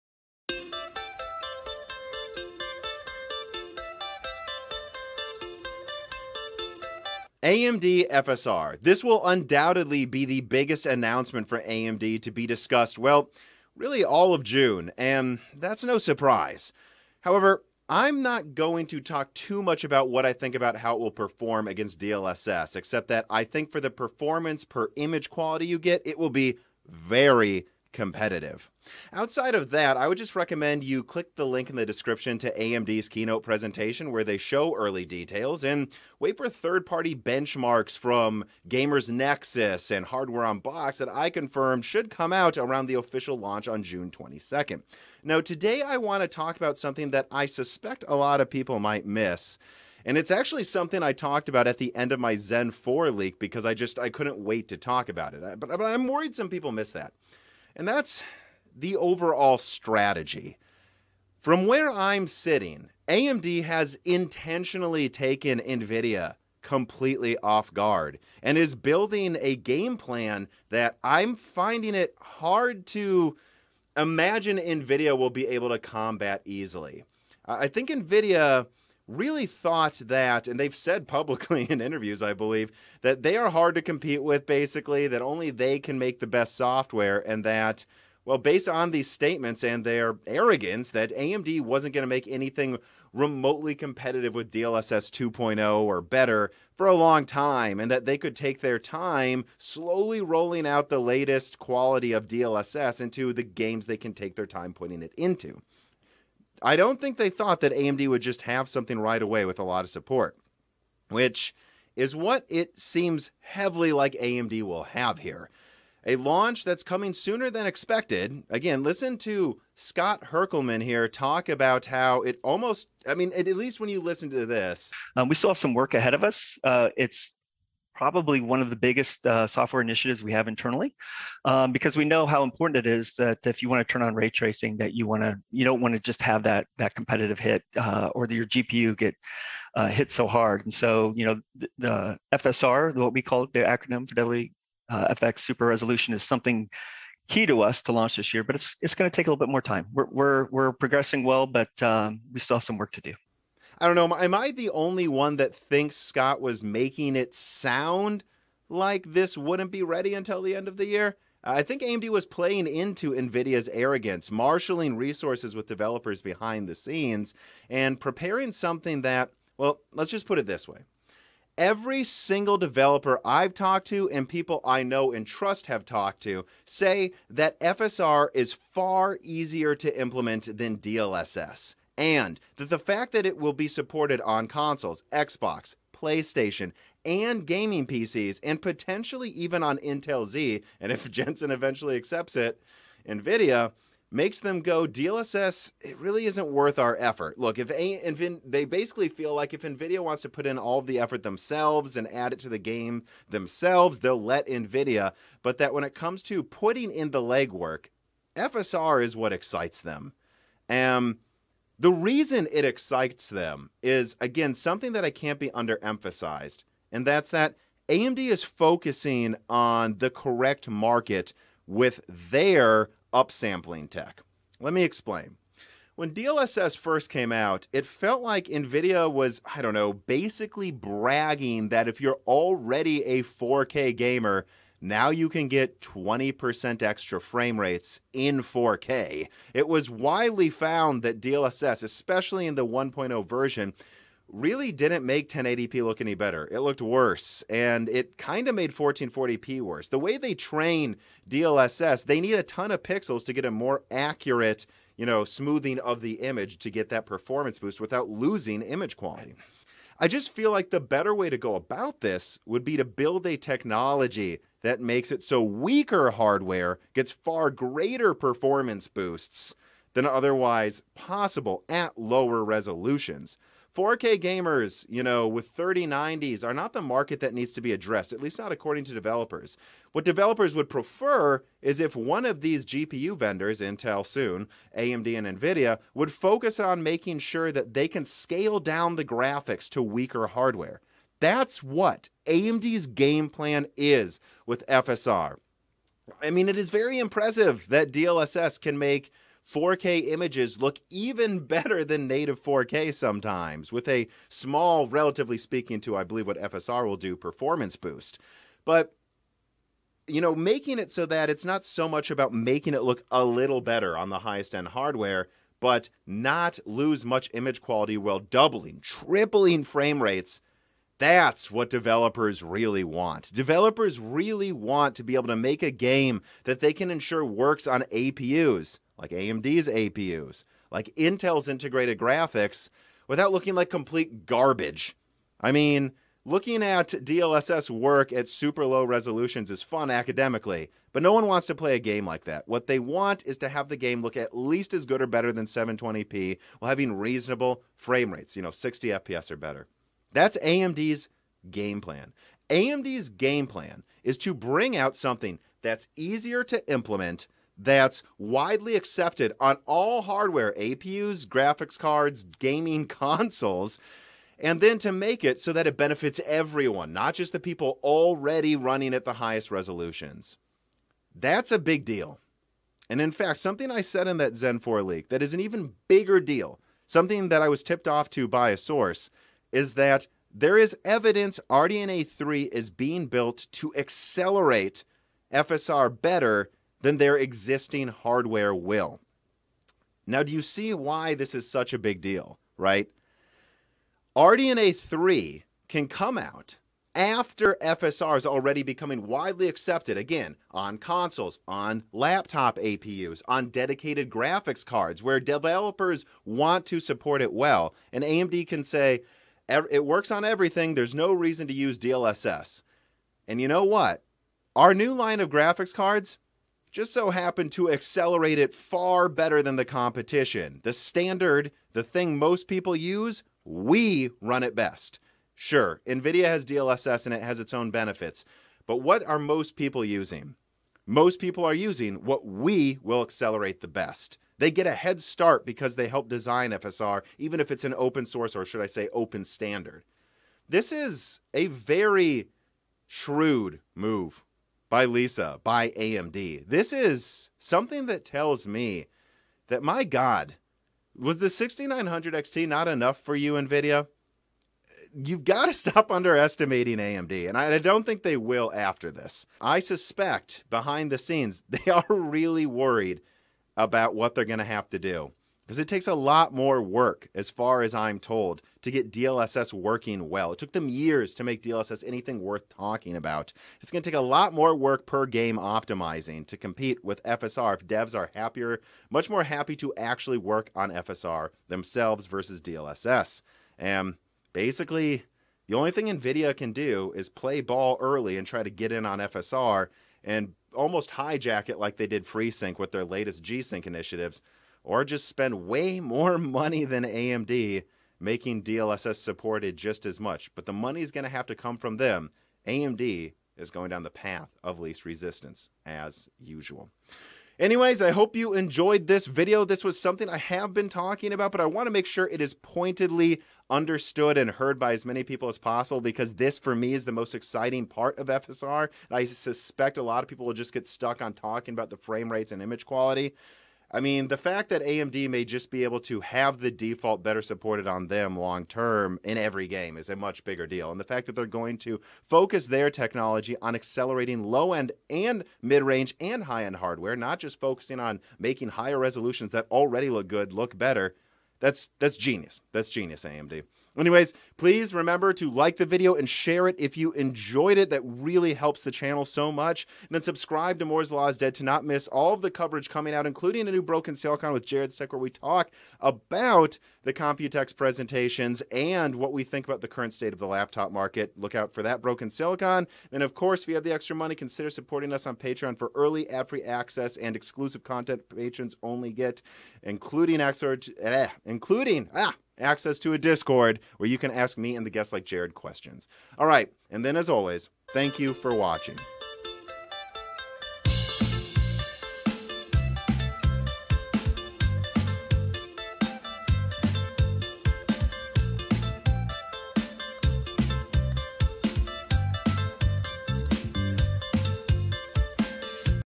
AMD’s DLSS Killing Strategy – Making FSR the Standard before RDNA 3 Accelerates it!
What would make RADEON FidelityFX Super Resolution truly successful? A well thought out strategy Nvidia didn’t see coming…

0:00 AMD likely jebaited Nvidia again…
3:34 FSR caters to the right market – Most gamers!
6:10 There are selfish reasons AMD wants FSR to be used by everyone…

Address Fan Mail to:
Moore's Law Is Dead,

Broken Silicon Logo by Tom.

2021 Video Intro by Tom & abstractrealism on the MLID Discord